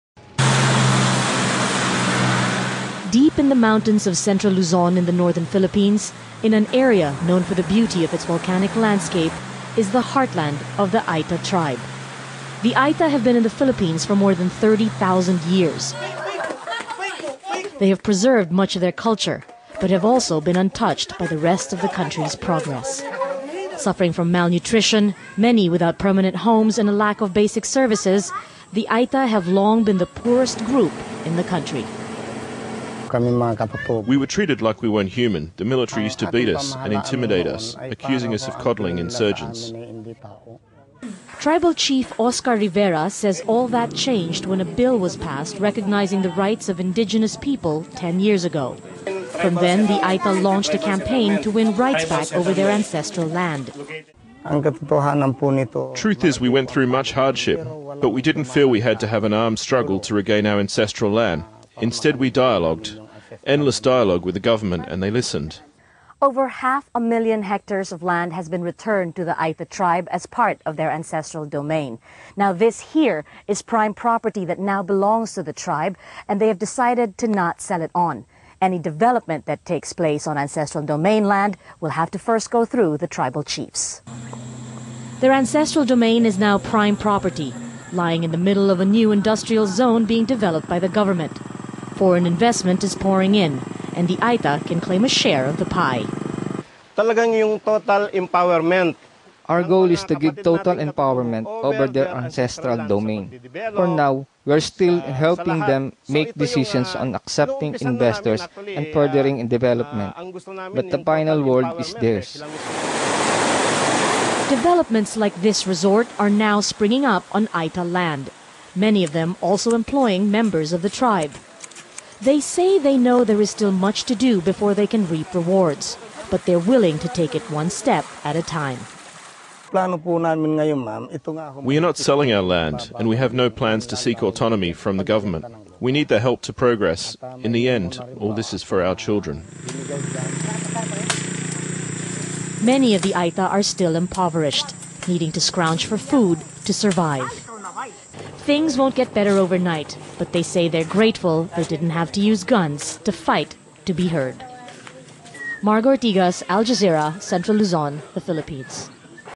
Aeta Struggle For Autonomous Land
The Aeta have been struggling for an autonomous homeland for decades, but indigenous tribes in central Luzon up north have gained recognition over their ancestral domain.